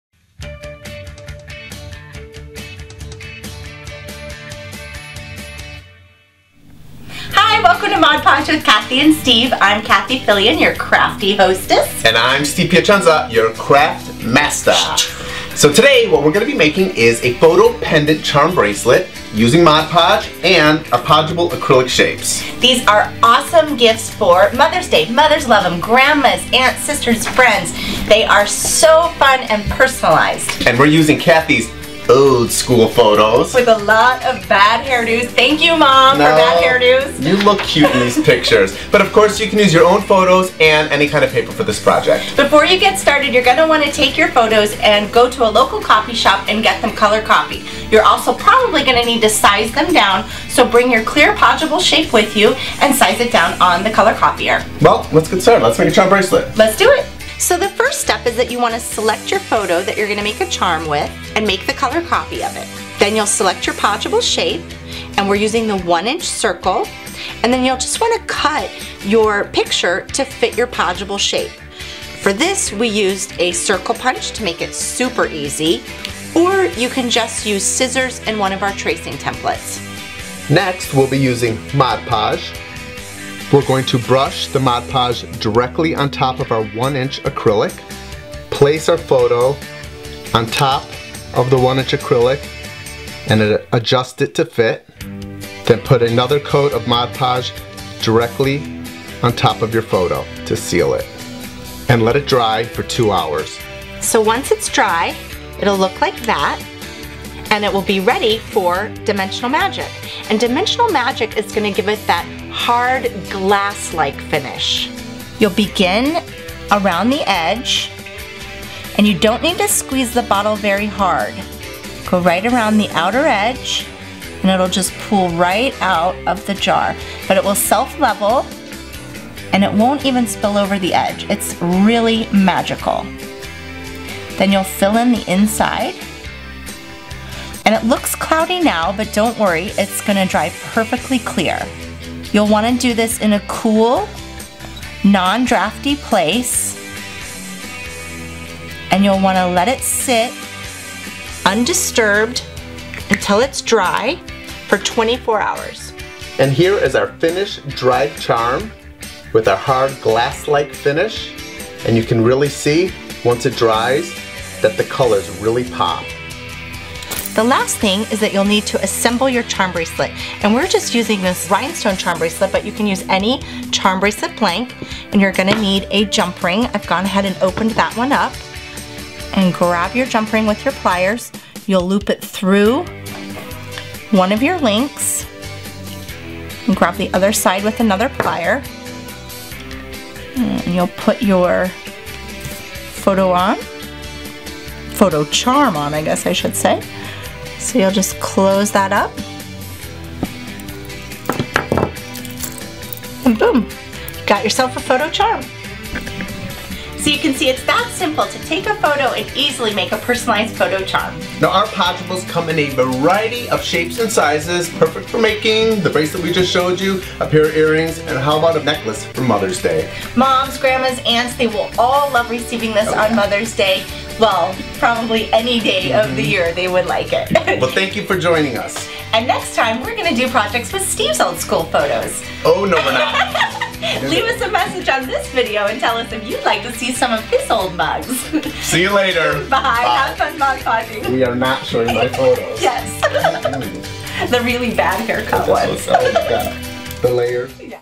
Learn How to Make a Photo Pendant Charm Bracelet
Watch as Cathie Filian and Steve Piacenza create a photo pendant charm bracelet using Mod Podge, podgeable shapes, and old photos. This charming charm bracelet would make a great gift for Mother's Day or any day you want to add a touch of sentiment to your craft projects.